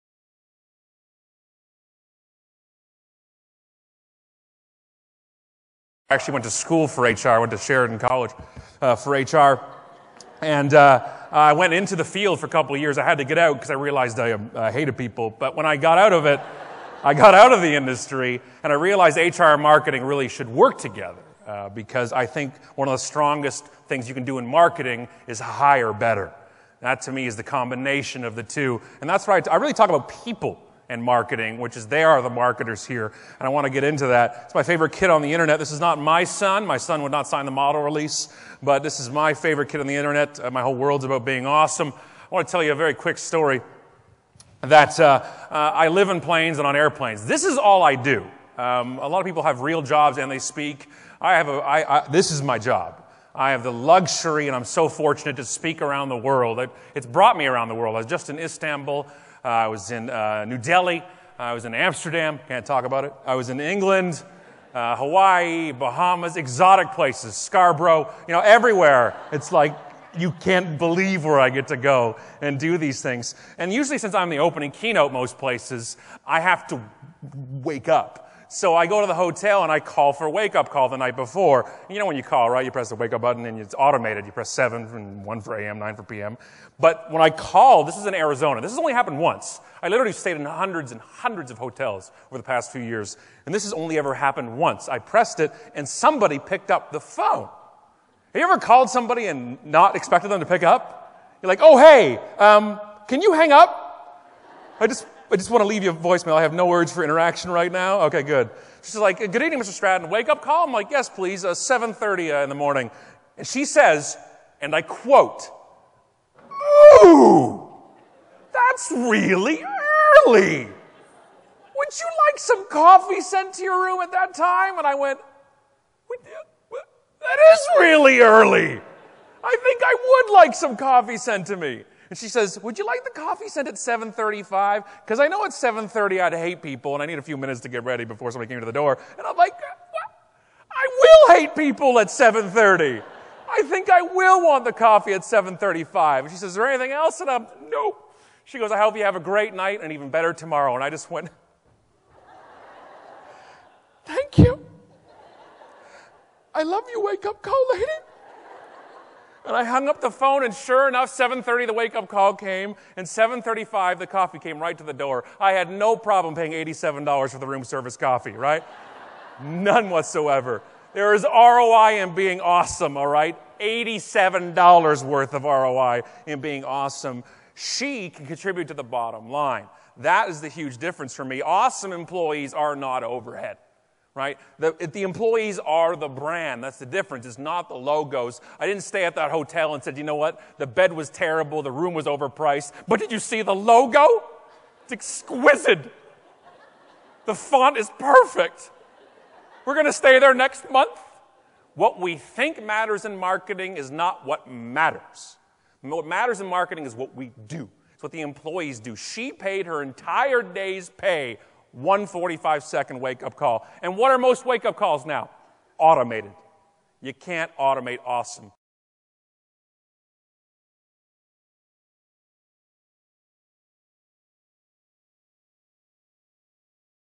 Scott Stratten - Speakers' Spotlight Showcase - Toronto
Scott Stratten - Expert in Un-Marketing

For generations, marketing has been hypocritical. We've been taught to market to others in ways we hate being marketed to―why do we still keep trying the same stale marketing moves? Scott Stratten, an expert in viral, social, and authentic marketing shows audiences (using real life, practical examples) how to unlearn the old ways and consistently attract, engage, and hold on to the right customers.

Formerly a music industry marketer, national sales training manager, and a professor at the Sheridan College School of Business, Stratten ran his "UnAgency" for a nearly a decade before solely focusing on speaking at events for companies like PepsiCo, Adobe, Red Cross, Cirque du Soleil, Saks Fifth Avenue, and Fidelity Investments. Stratten has over 135,000 people who follow him on Twitter, he was named one of the "Top Five Social Media Influencers in the World" by Forbes.com, and named one of "America's 10 Marketing Gurus" by Business Review USA.